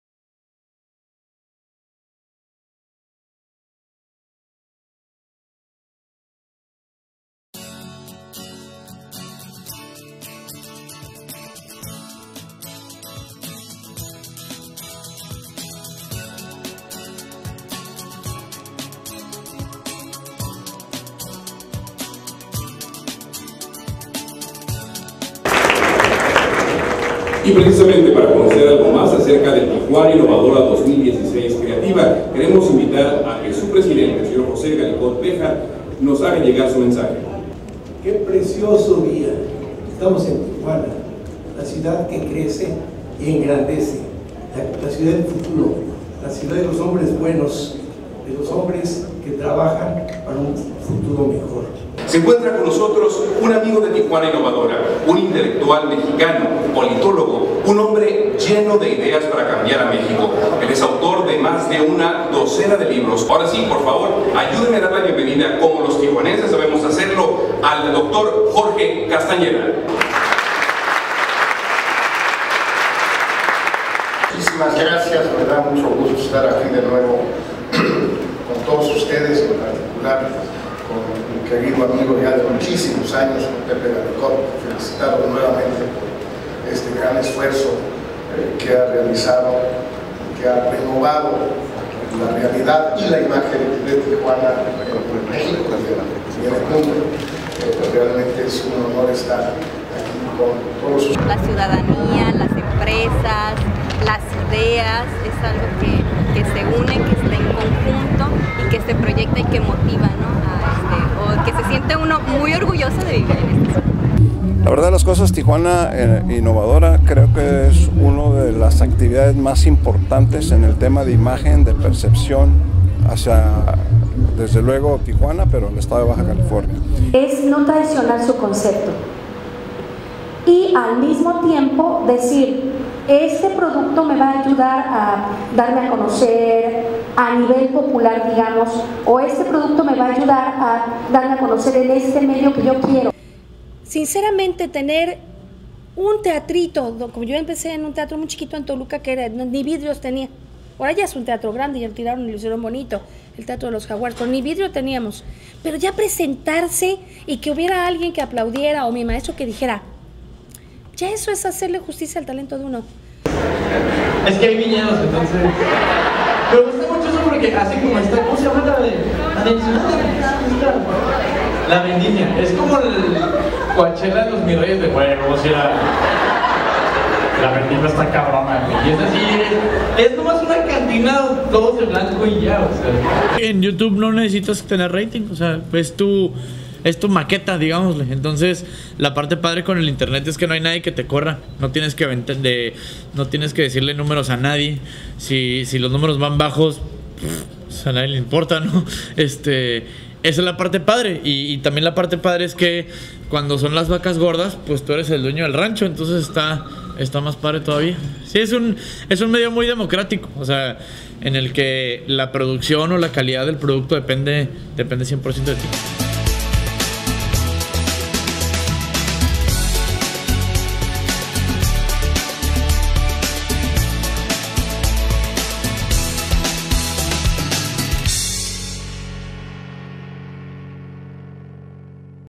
Dia1 TI Inauguración 3 0ct.2016
Arranca Tijuana Innovadora 2016 con un enfoque hacia las industrias creativas, la moda y la sustentabilidad en su cuarta edición. Inaugurado por el gobernador del estado Francisco Vega, el presidente municipal Jorge Aztiazarán entre otras autoridades importantes. Con la participación del político y escritor Jorge Castañeda, la nominada al premio de la Academia Adriana Barraza y el famoso youtuber mexicano Chumel Torres en el primer día de actividades.